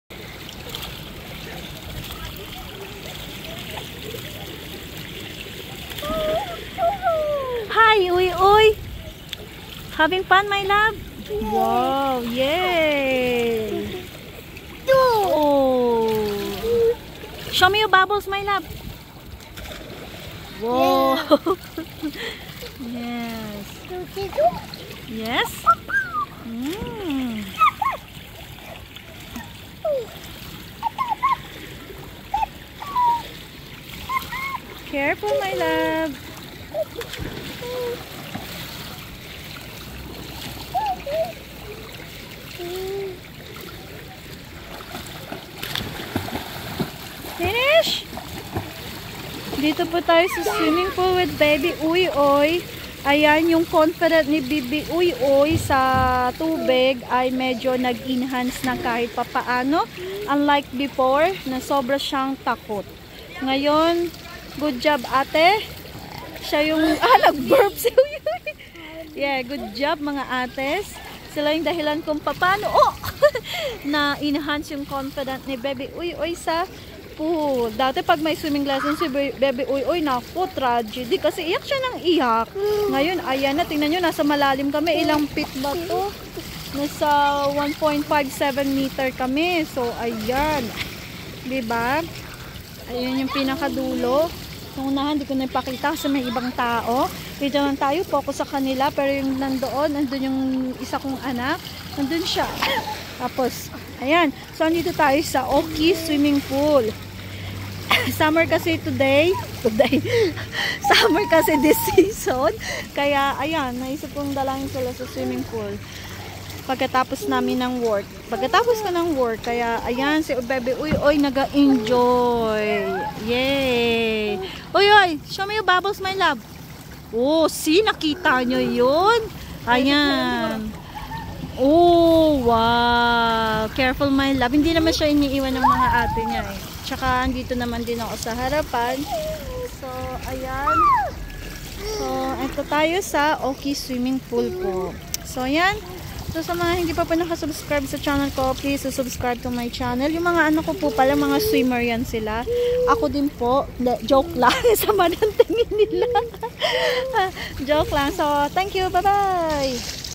Three year's old building water confidence.
It's summer season here in Australia, let's go swimming with baby Oi-oi and with mga sissy.